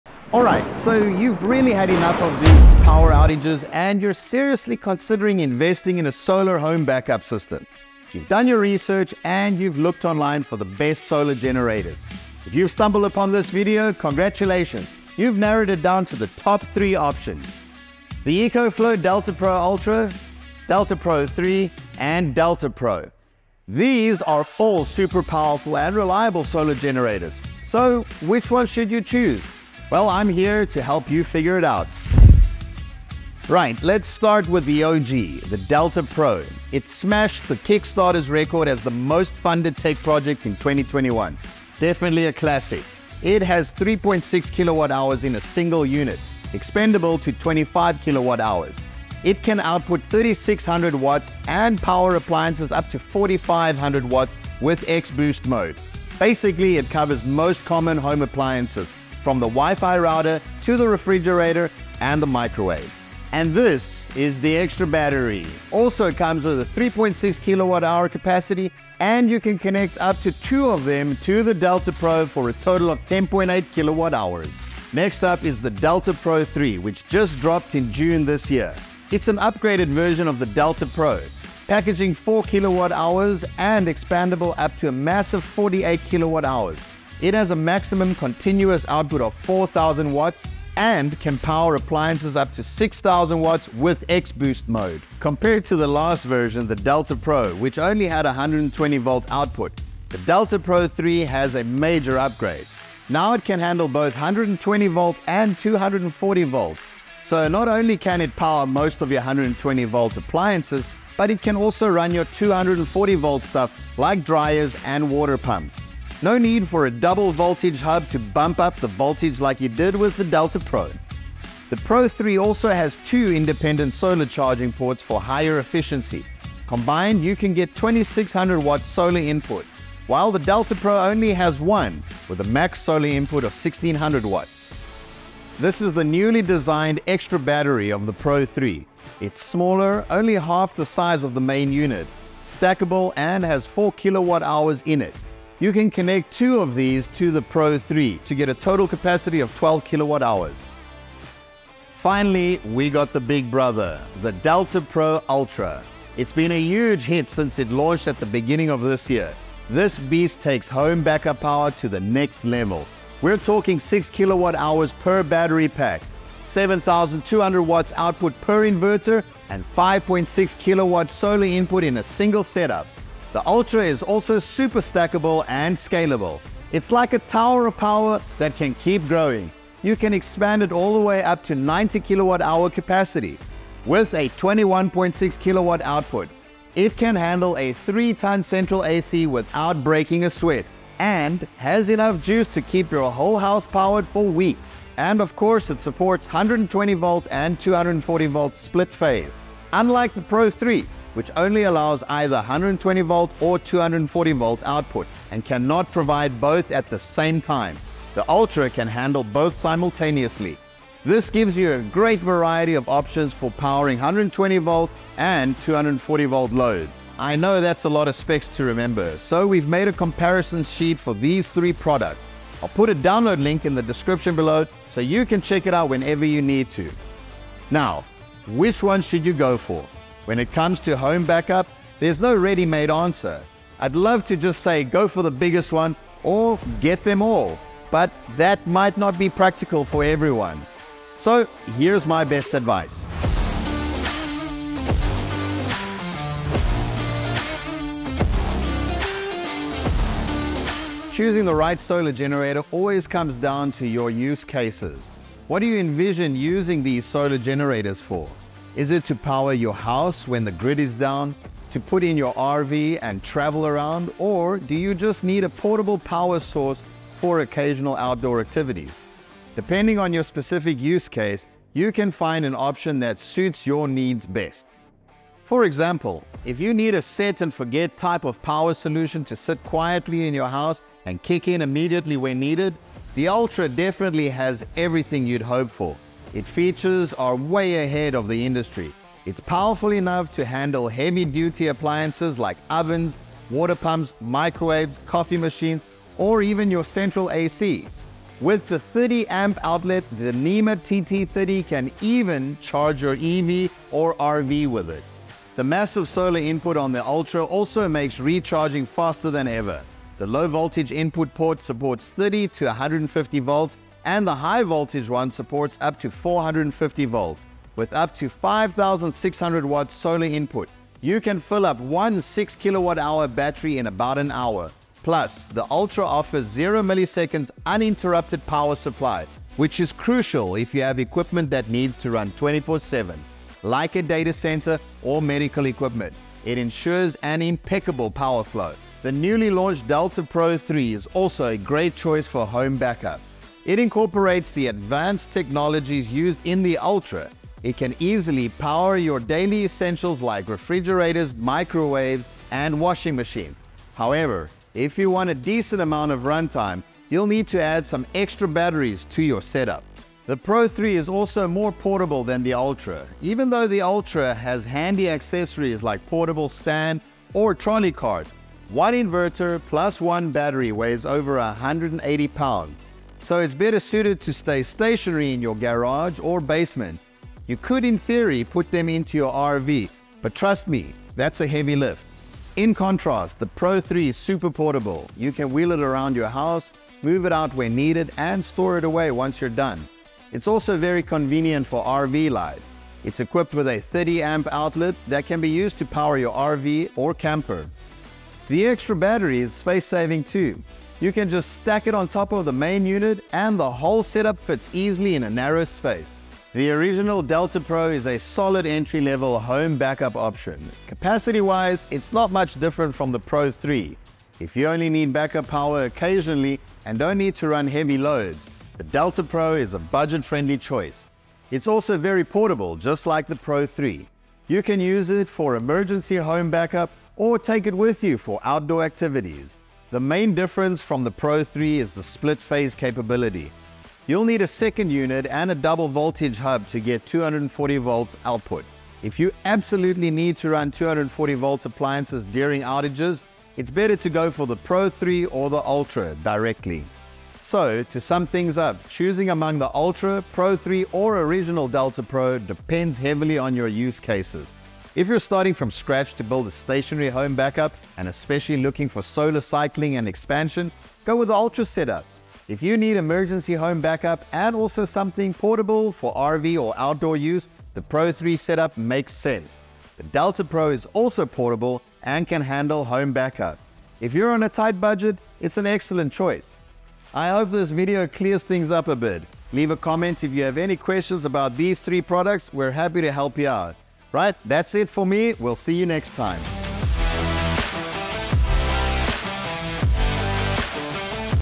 Choosing the Right EcoFlow DELTA: the Pro, the 3, the Ultra?
Choosing the right backup power solution doesn't have to be tricky. Our latest guide breaks down the features of EcoFlowDELTAPro3, EcoFlowDELTAProUltra, and EcoFLowDELTAPro, helping you see which one best meets your needs.

📌Timestamps
00:00 Intro
00:35 EcoFlow DETLA Pro's Feature
01:15 EcoFlow DELTA Pro 3's Advantage
02:32 EcoFlow DELTA Pro Ultra's Edge
03:49 Which one is right for you
07:28 Outro